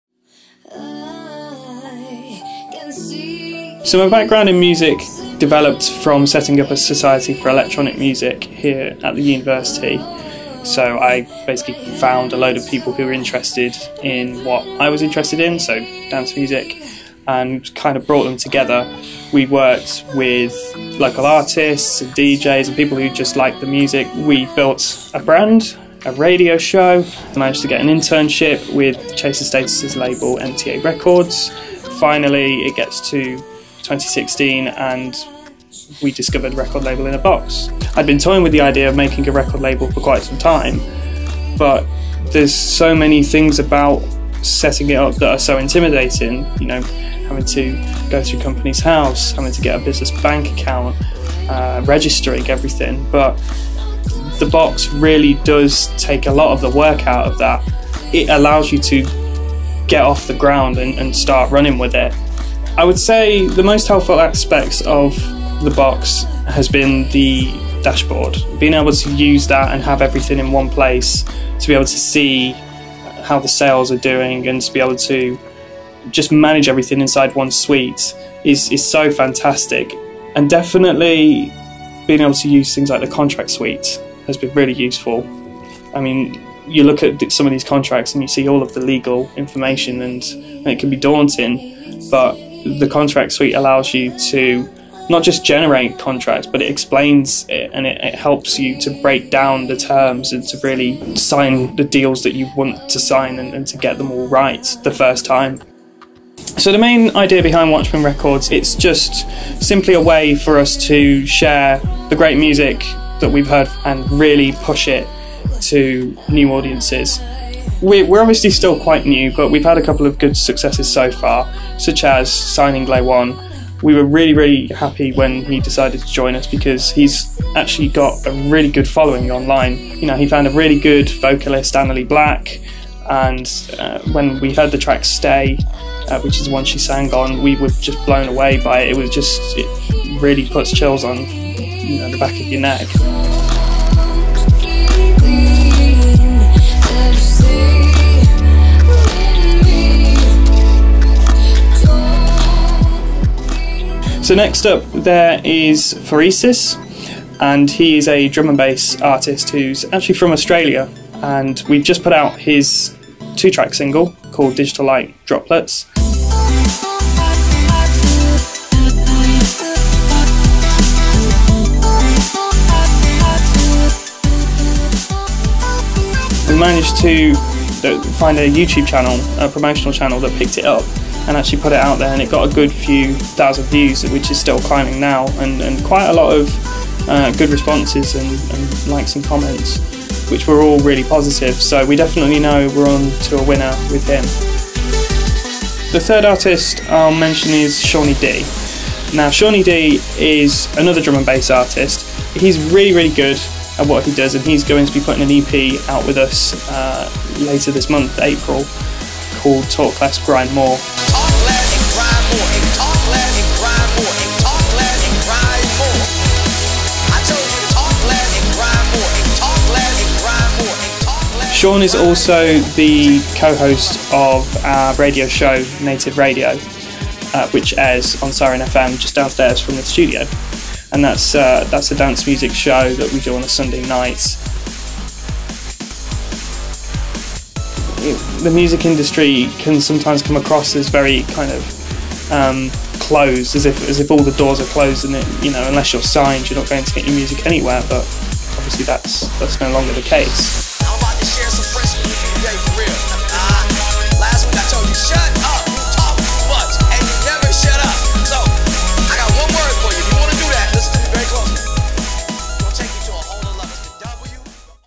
How To Start A Record Label // Watchman Records
We speak to Martyn, the founder of Watchman Records about how he started their record label using Record Label in Box, the artists they have signed and the obstacles they have overcome in setting up.